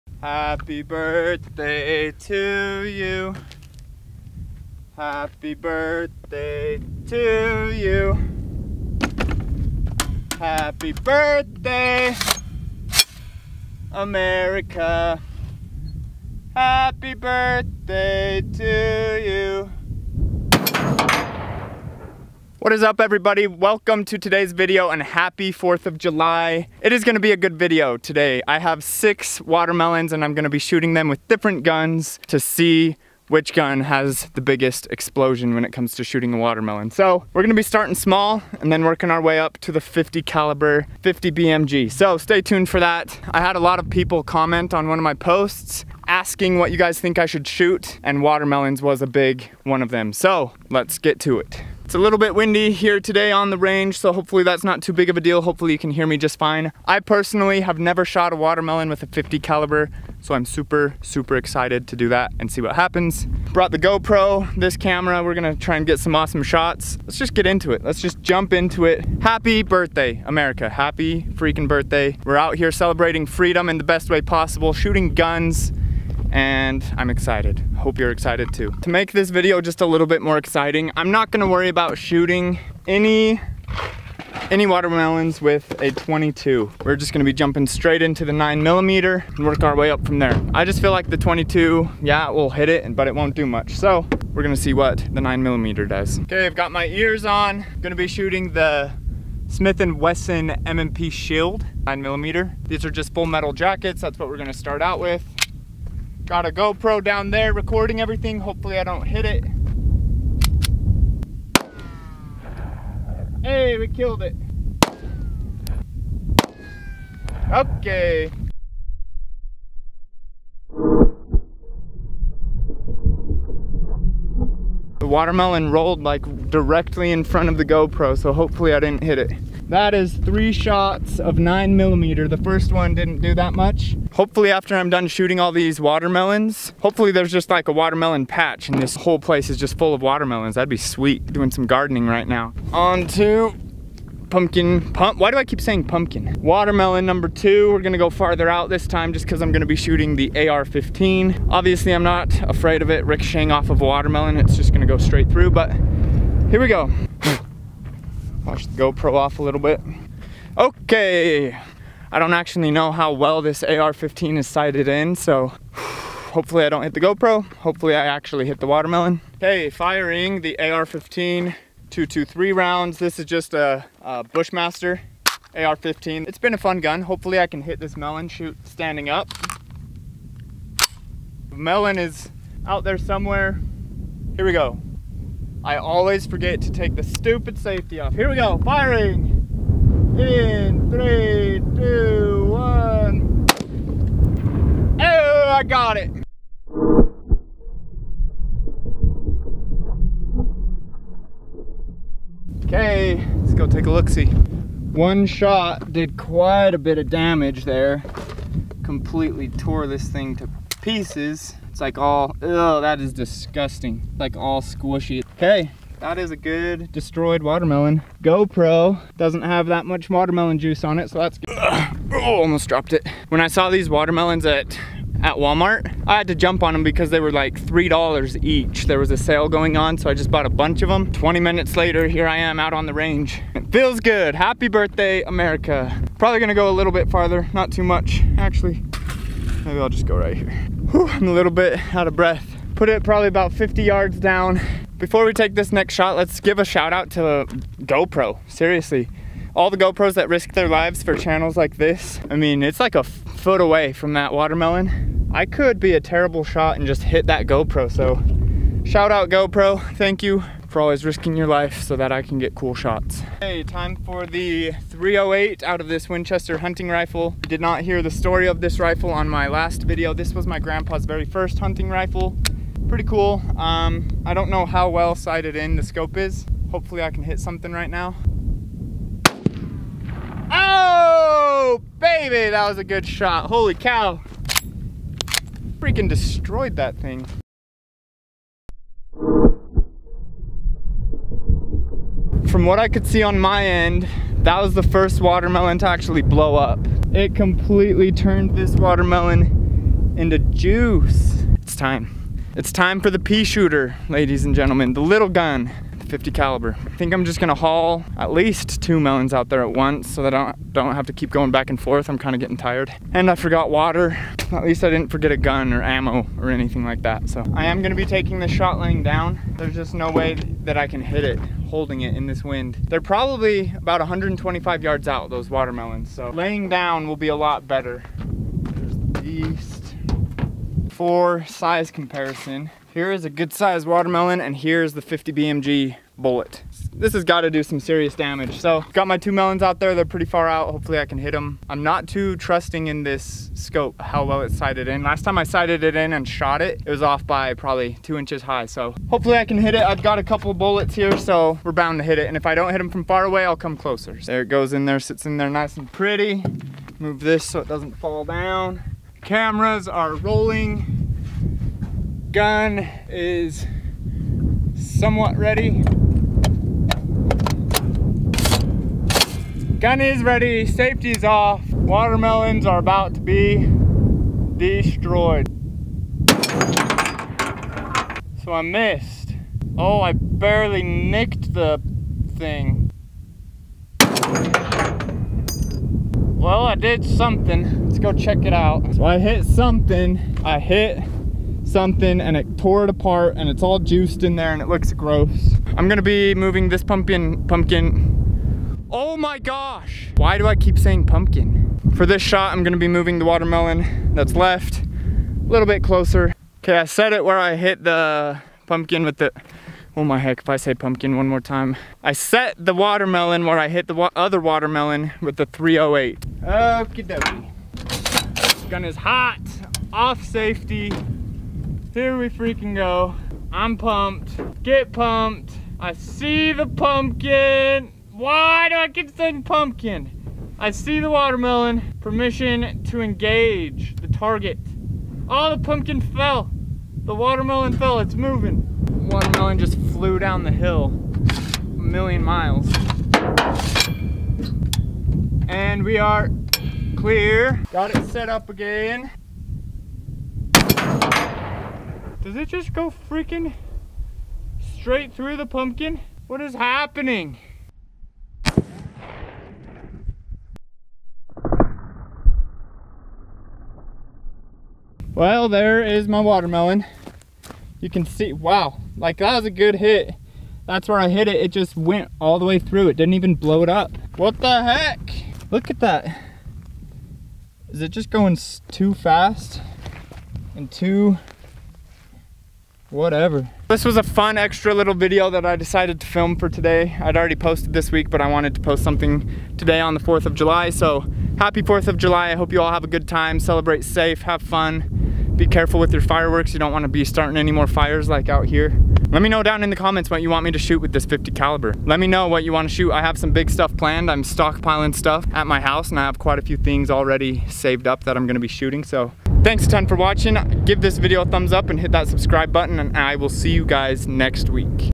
Can Watermelons Stop A 50cal?
Can watermelons stop a 50bmg? To celebrate the 4th of July, I decided to go out to the range and shoot some watermelons. I shot the melons with different guns so you can see which ones have the biggest explosion! Watch the entire video to see watermelons vs 50cal.